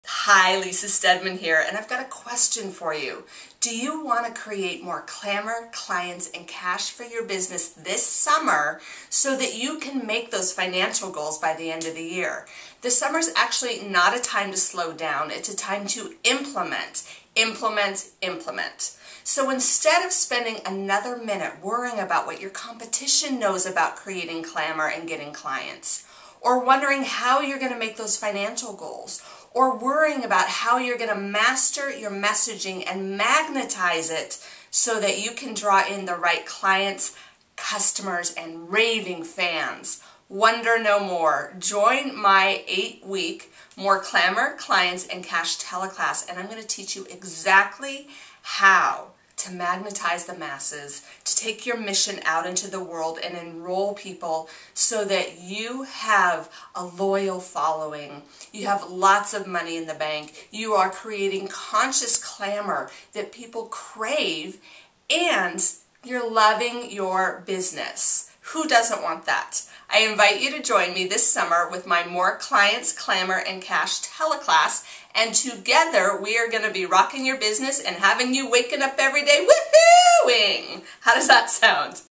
Want MORE Clamor, Clients, & Cash?
This summer want MORE clamor clients and cash? Don't miss your chance to uplevel your business by perfecting your rock star social brand with Chief Woohoo Woman and Clamor Queen Lisa Steadman.